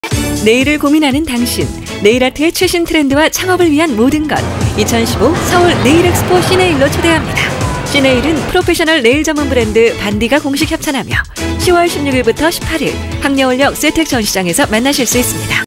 2015 SINAIL COMMING SOON
2015년 시네일!
네일아트의 최신 트렌드와 창업에 관련한 모든 것을 한 곳에서, 한 눈에 확인 할 수 있는 곳 !
국내 최대의 2015년 네일 박람회를 놓치지 마세요!